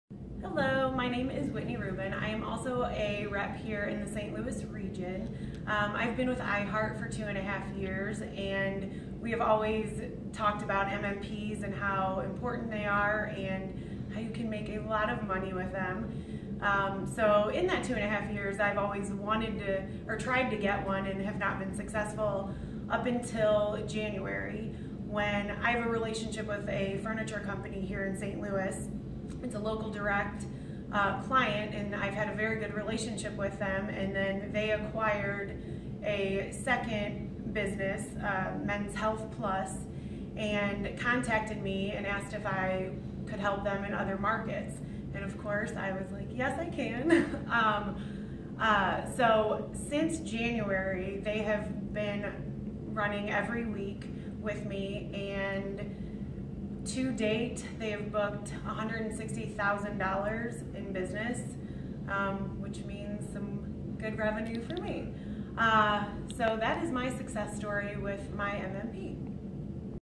Whitney Rubin 2019 MMP Success
iHeartMedia Account Executive discussing success with Multi Market Partnerships.